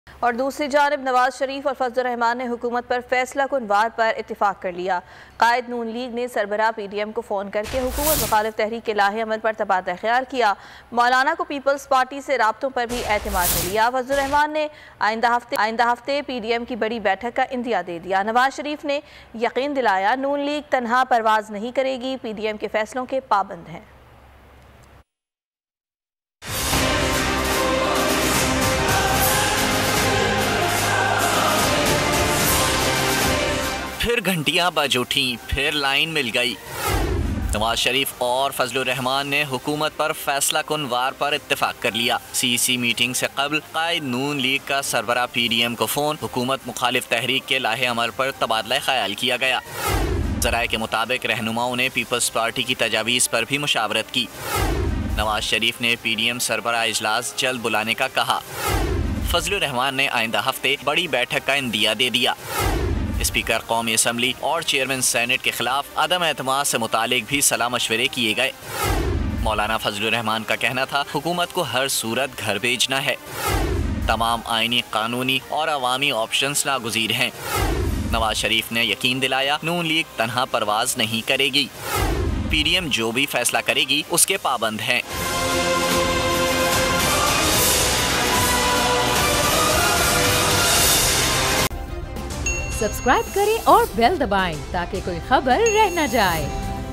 PMLN Ka Tanha Parwaz Na Karne Ka Irada | Nawaz Sharif calls Fazlur Rehman | Breaking News
Nawaz Sharif | Fazal ur Rehman | Nawaz Sharif Calls Fazal ur Rehman | Nawaz Calls Fazal | Breaking News | BOL News